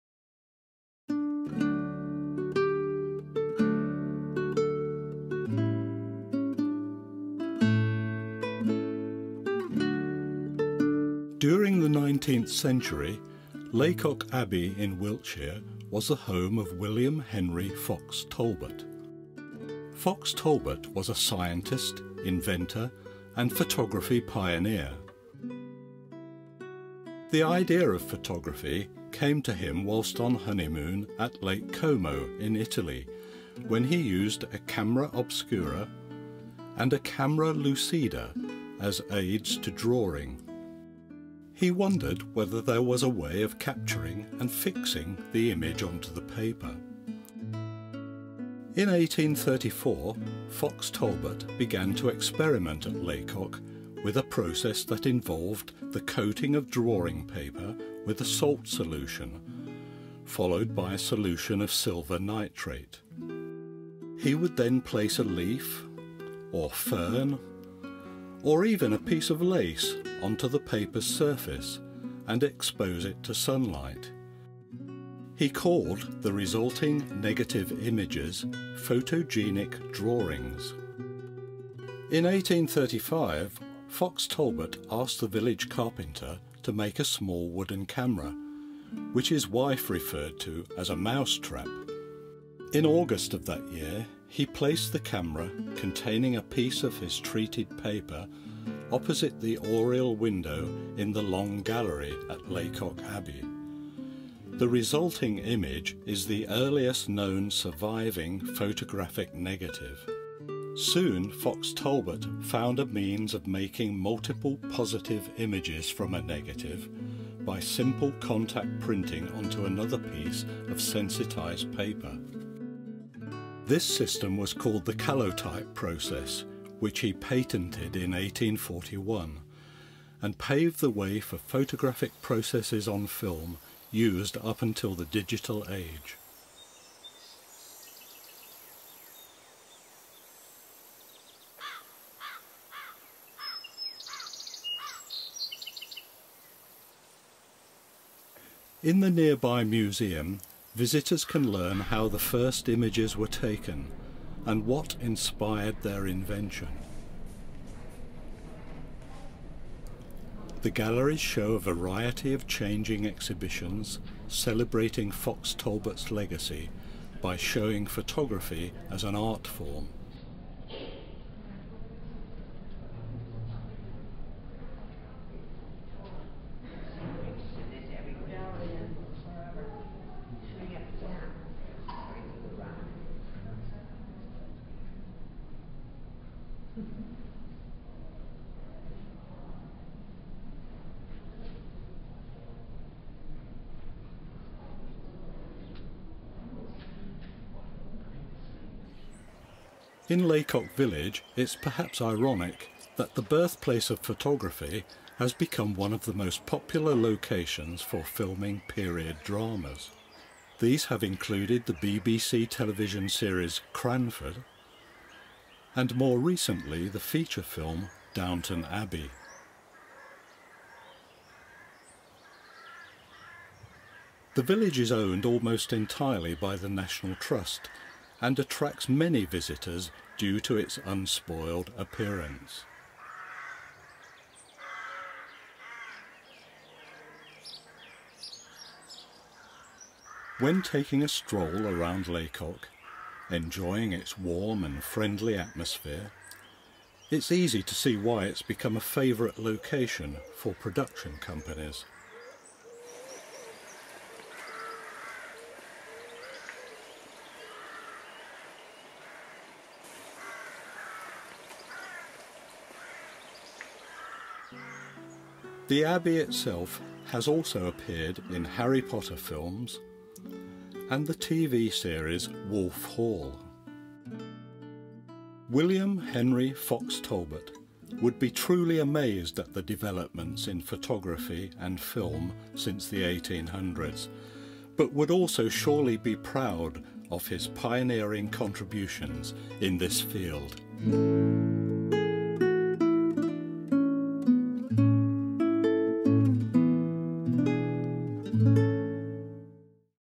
Britain's Birthplace of Photography
The parts played by Lacock Abbey and William Henry Fox Talbot in the development of photography. Also a look at the lovely village of Lacock in Wiltshire, which has often appeared as a location in film and television productions.